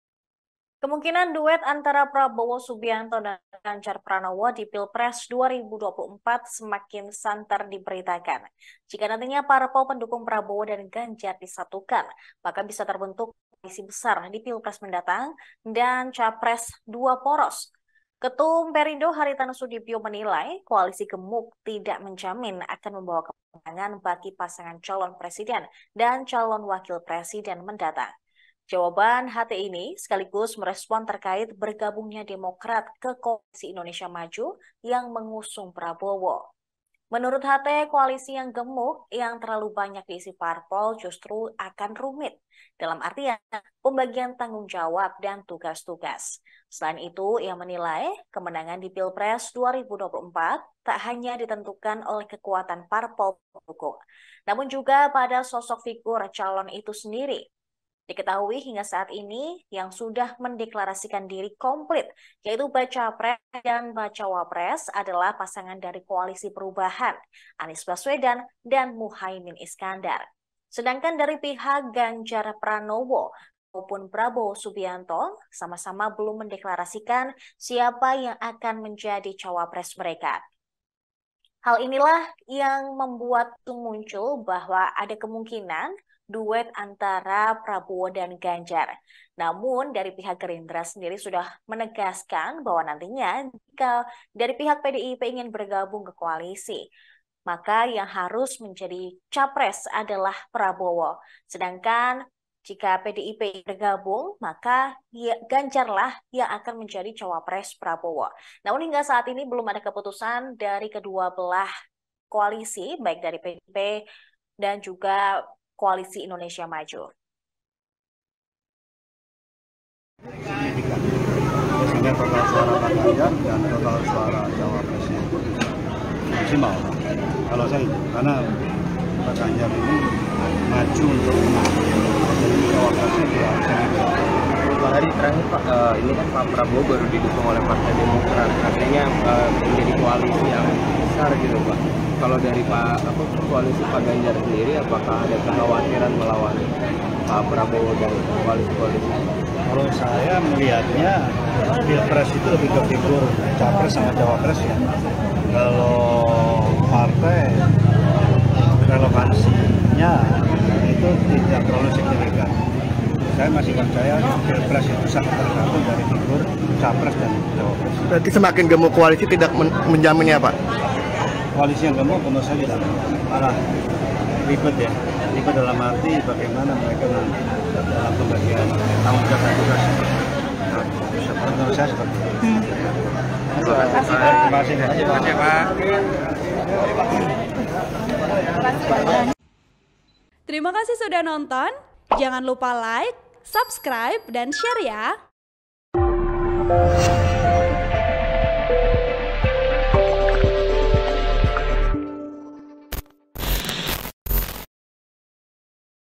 HT Soal Duet Prabowo-Ganjar: Koalisi Gemuk Tak Menjamin Kemenangan, Malah Ribet!
TRIBUN-VIDEO.COM - Kemungkinan duet antara Prabowo Subianto dan Ganjar Pranowo makin santer diberitakan.

Jika nantinya parpol pendukung Prabowo dan Ganjar disatukan, maka bisa terbentuk koalisi besar di Pilpres 2024.

Ketum Perindo Hary Tanoesudibjo (HT) menilai, koalisi gemuk tidak menjamin akan membawa kemenangan bagi pasangan calon presiden dan calon wakil presiden mendatang.

Jawaban HT ini sekaligus merespon terkait bergabungnya Demokrat ke Koalisi Indonesia Maju yang mengusung Prabowo.

Menurut HT koalisi yang gemuk yang terlalu banyak diisi parpol justru akan rumit.

Dalam artian pembagian tanggung jawab dan tugas-tugas.

Selain itu ia menilai kemenangan di Pilpres 2024 tak hanya ditentukan oleh kekuatan parpol pendukung.

Namun juga pada sosok figur calon itu sendiri.

Diketahui hingga saat ini yang sudah mendeklarasikan diri kompit bacapres dan bacawapres adalah pasangan dari Koalisi Perubahan, Anies Baswedan dan Muhaimin Iskandar (Cak Imin).

Sedangkan dari pihak Ganjar maupun Prabowo sama-sama belum mendeklarasikan siapa yang akan menjadi cawapres.

Ada kabar yang beredar soal kemungkinan duet Prabowo dan Ganjar.

Namun beberapa pakar politik menilai hal ini sulit terealisasi karena kedua parpol pengusung yaitu PDIP dan Partai Gerindra sama-sama memiliki suara yang kuat dan historis yang melatarbelakanginya.

Host: Saradita
VP: Indra